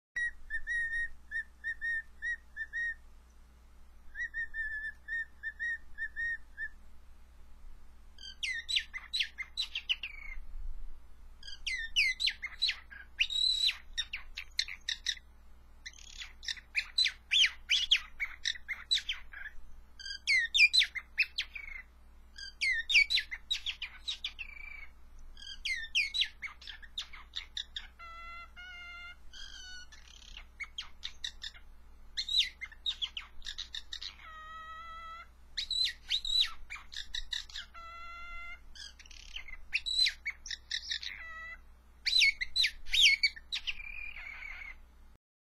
Nuggets Bird Like Song
Nugget singing a collection of bird like sounds